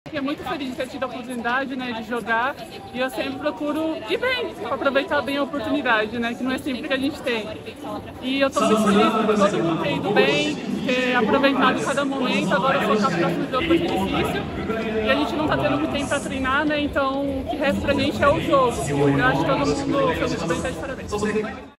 DEPOIMENTO | DIANA | PRÉ-OLÍMPICO FEMININO | 20/09/2023
A central Diana, com 13 acertos, contribuiu bastante para a vitória da seleção feminina de vôlei sobre Porto Rico por 3 sets a 0, pelo Pré-Olímpico de Tóquio, no Japão. Confira o depoimento da jogadora.